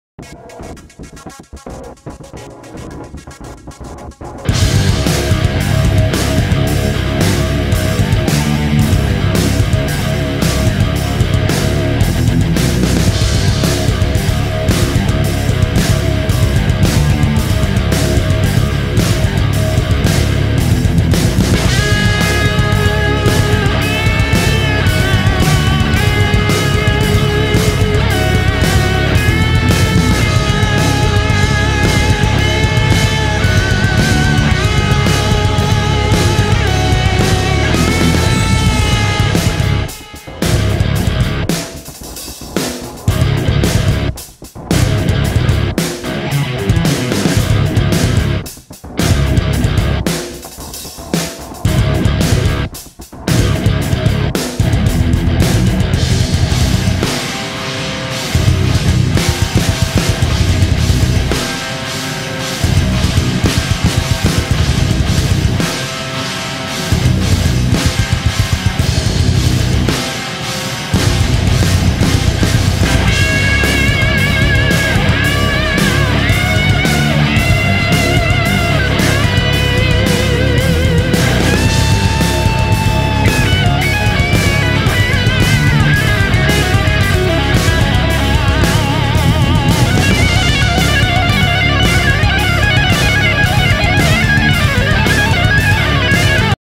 S.H.Monsterarts - Godzilla 2019 & King Ghidorah 2019 ゴジラ2019 & キングギドラ2019
Another look at the S.h.monsterarts Godzilla 2019 & King Ghidorah 2019